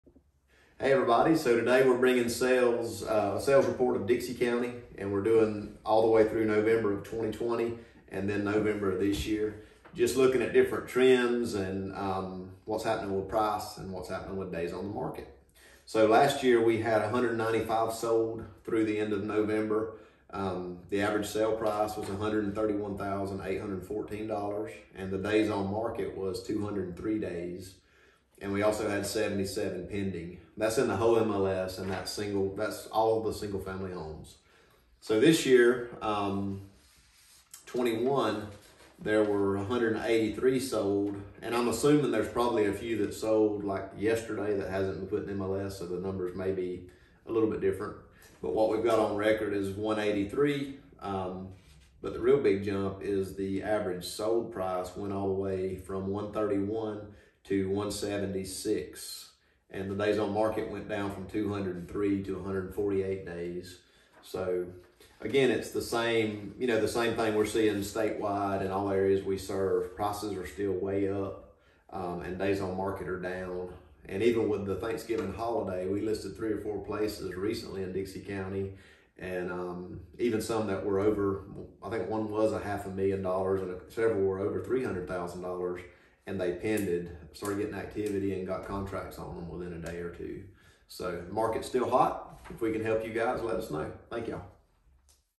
Market update Dixie county 2020/2021
This year has been an amazing year for buyers and sellers. Interest rates have been super low and home equity is still very high. Days in market have also remained much shorter too. We decided to look at year over year to date and the numbers do not lie. It is a great time to sell!